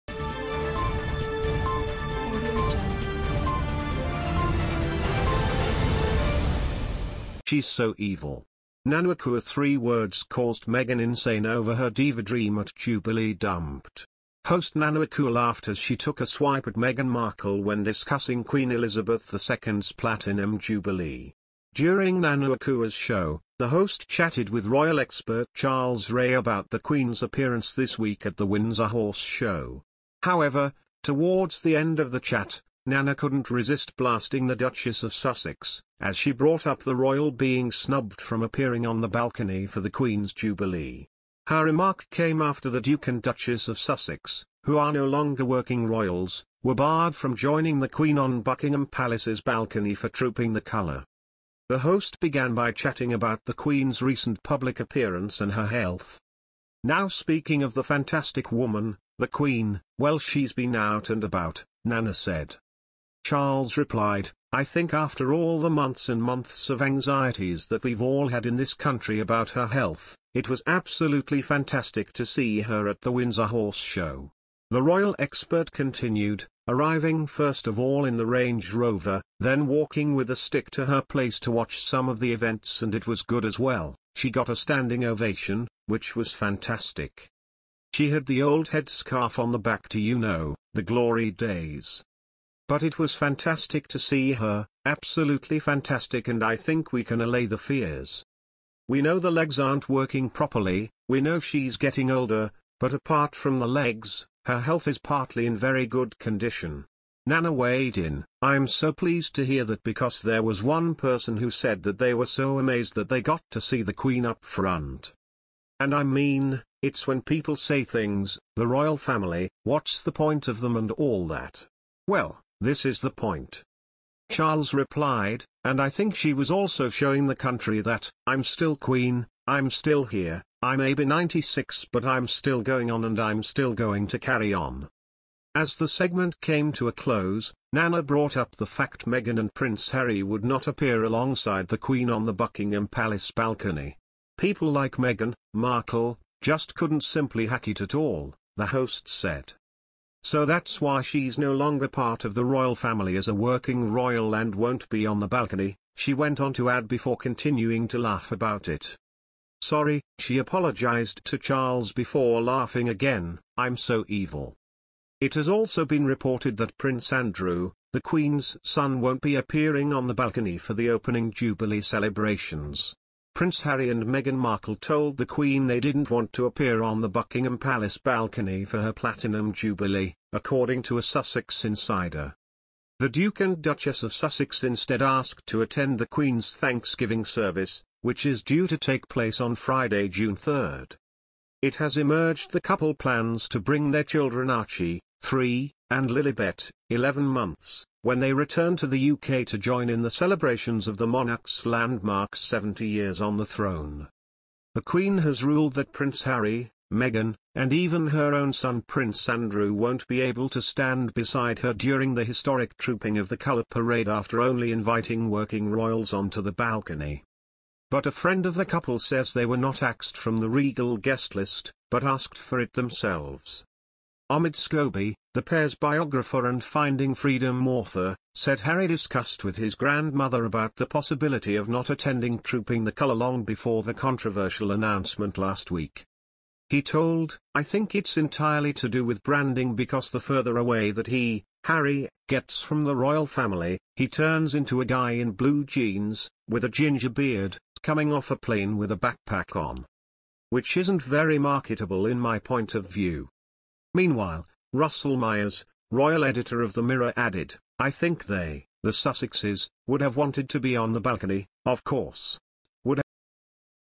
'She’s So EVIL'! Nana Akua Three Words Caused Meghan INSANE Over Her Diva Dream At Jubilee DUMPED
Charles replied: "I think after all the months and months of anxieties that we've all had in this country about her health, it was absolutely fantastic to see her at the Windsor Horse Show.
The royal expert continued: "Arriving first of all in the Range Rover, then walking with a stick to her place to watch some of the events and it was good as well, she got a standing ovation, which was fantastic.

"She had the old headscarf on the back to you know, the glory days.

"But it was fantastic to see her, absolutely fantastic and I think we can allay the fears.